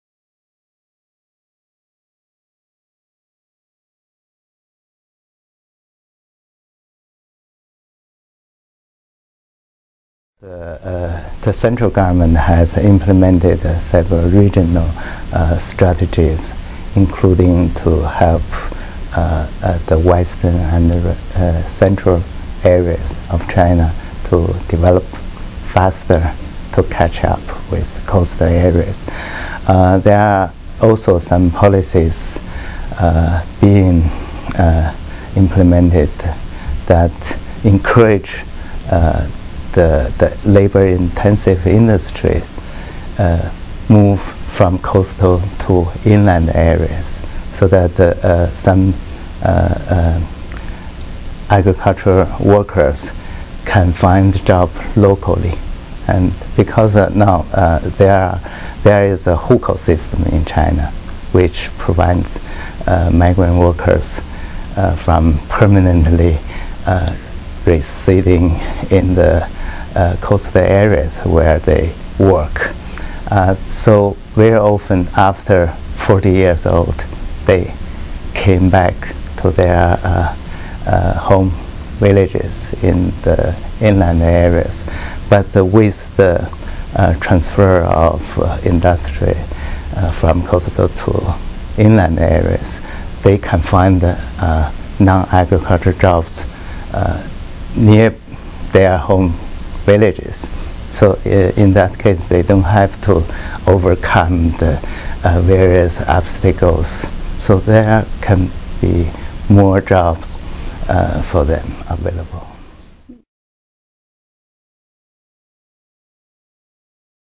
Balancing economic growth within China (2/2)
Cai Fang, Director of the Institute of Population and Labor Economics at the Chinese Academy of Social Sciences, discusses the challenge of balancing economic growth within China.

He says the Central Government has created several policies to address this discrepancy, primarily a set of regional strategies to help the slower-developing western and central areas to create jobs and catch up to the country's booming coastal regions.

Fang also notes policies being implemented to encourage labor-intensive industries to move inland from the coastal areas to help agricultural workers find jobs closer to their home villages. This policy seeks to address the problem created by China's "hukou" system, which prevents migrant workers from permanently residing in the coastal areas where they work. Until now, this created a situation where workers returning home faced difficulty finding good jobs.